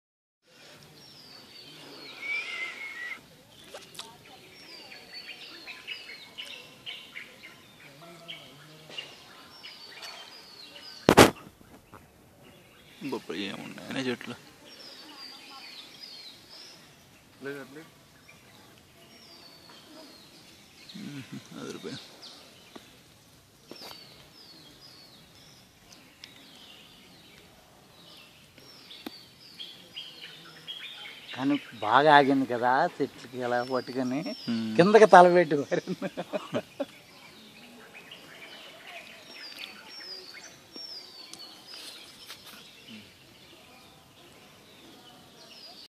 Red Squirrel eating coconut
At japali theerdham, Thirupathi.